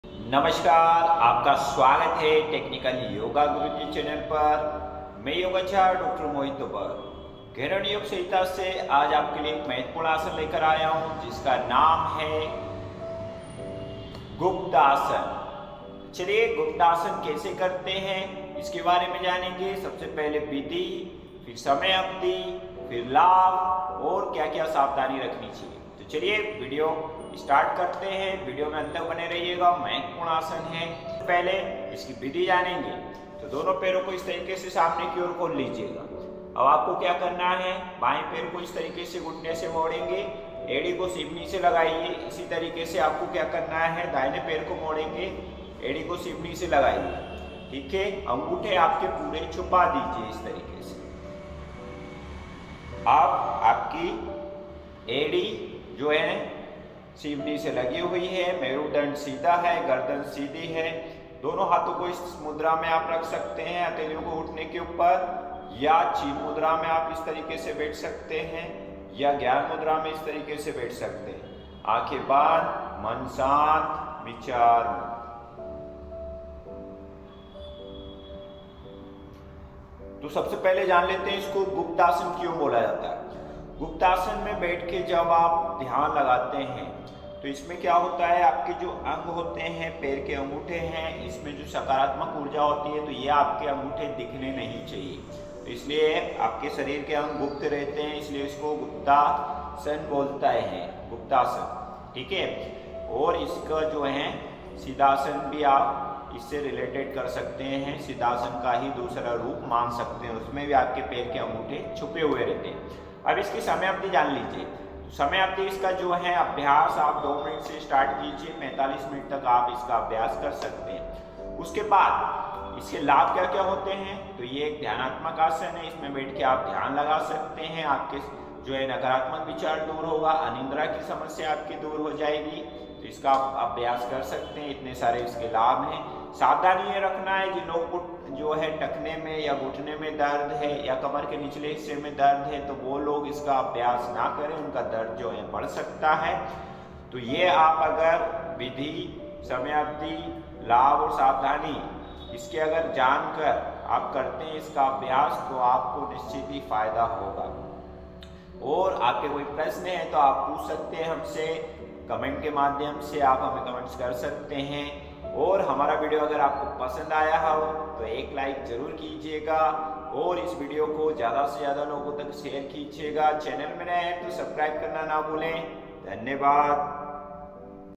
गुप्तासन का रहस्य जान लो क्या है | Guptasana Benefits | Technical Yoga Guruji
Hi
 I Am Dr.Mohit Kumar Tawar. Welcome to the Our YouTube Channel Technical Yoga Guruji

About this video-:
Hope you will enjoy the video & implement the knowledge of yoga in your daily life

गुप्तासन का रहस्य जान लो क्या है | Guptasana Benefits

घेरण्ड योग संहिता
से आपके लिए
एक महत्वपूर्ण
आसन लेकर आया हूँ
जिसका नाम है
गुप्तासन

गुप्तासन
की
विधि
समय अवधि
लाभ
और
सावधानियां

इसको गुप्तासन क्यों
बोलते हैं
सबसे पहले जान लेते हैं
क्योंकि इस आसन में
आपके पैरों के पंजों
को छुपाना होता है
इस लिये इसका
नाम गुप्तासन
रखा गया है

विधि
सबसे पहले अपने
आसन पर बैठ
जायें पैरों को
सामने की ओर
खोल कर अब
बायें पैर को घुटने
से मोड़कर एड़ी को
सीवनी से लगा लें
अब दायें पैर को
घुटने से मोड़कर
एड़ी को बायें एड़ी
के ऊपर रख लेना है
पैर के पंजों को जंघा
के नीचे छुपा के रख
लेना चाहिए मेरुदण्ड
सीधा गर्दन सीधी
आँखों को बंद कर
बैठ जायें अब
हाथो को चिन
या ज्ञान मुद्रा में
घुटनो पर रख लें
 ये लग गया
गुप्तासन

समय अवधि
2 मिनिट से
शुरुआत करें
धीरे धीरे समय
बढ़ाकर 45 मिनिट
तक अभ्यास
कर सकते हैं

लाभ
1.ध्यान के लिए
महत्वपूर्ण आसन है
2.नकारात्मक विचारों
को दूर करता है
3.अनिंद्रा की
समस्या दूर हो
जाती है

सावधानी
टखनों में
या
घुटनो में
या
कमर के निचले
हिस्से में दर्द
होने पर इस
आसन का
अभ्यास ना करें
दर्द बढ़ सकता है